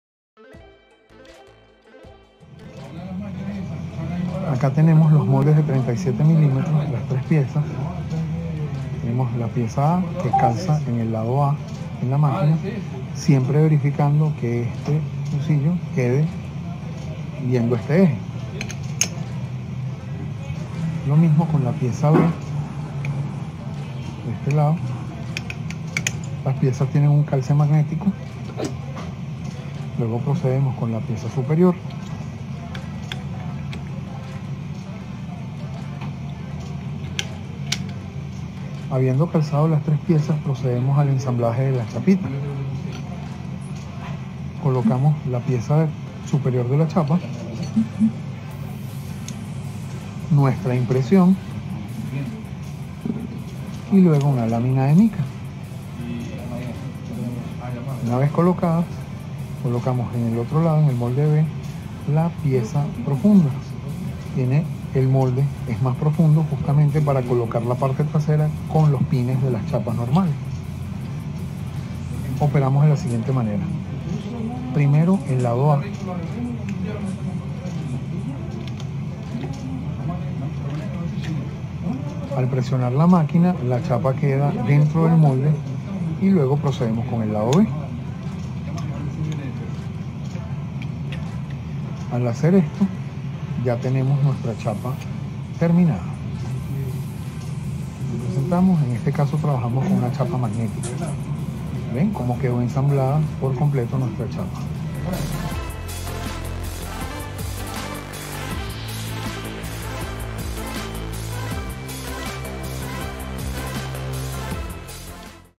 Chapita TMJ Molde de 37 mm
Paso a paso con TIMG

Veremos como usar la maquina de hacer Chapita modelo TMJ con molde de 37 mm.